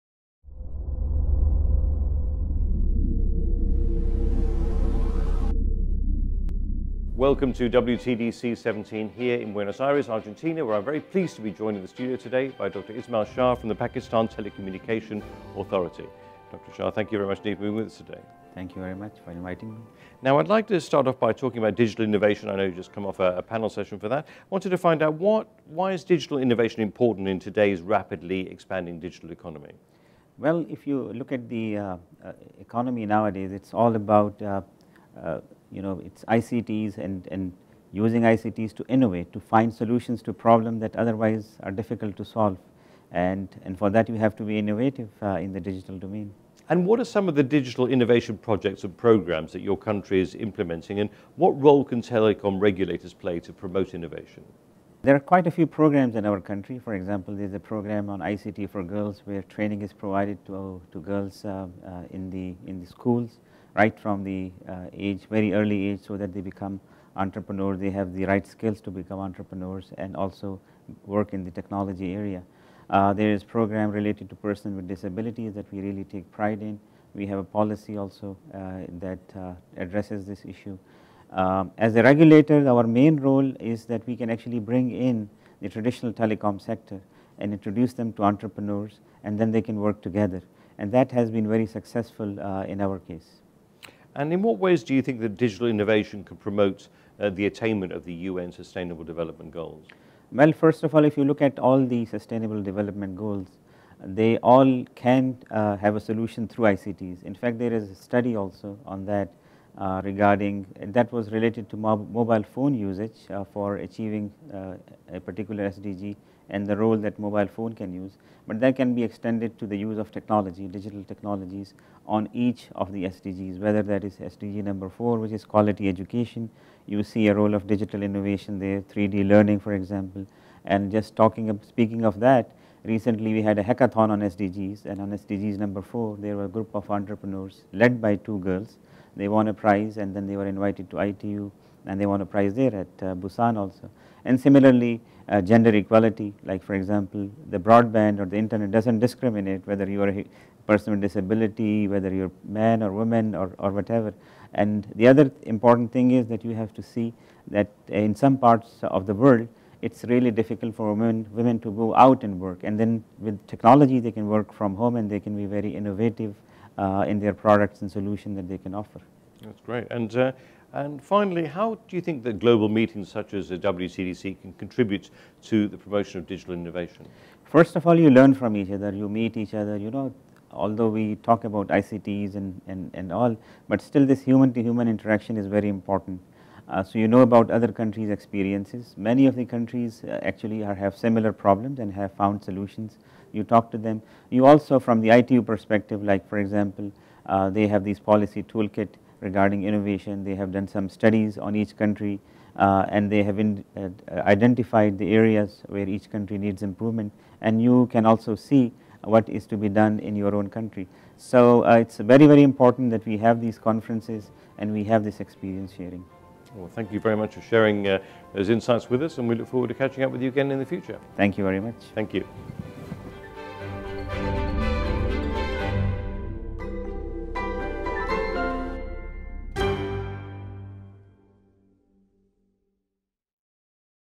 ITU INTERVIEWS @ WTDC-17: Syed Ismael Shah PH.D., Chairman, Pakistan Telecomm Authority, Pakistan
Interview with PhD Syed Ismael Shah, Chairman, Pakistan Telecommunications Authority, Pakistan at the World Telecommunication Development Conference (WTDC-17), Buenos Aires, Argentina, 9- 20 October 2017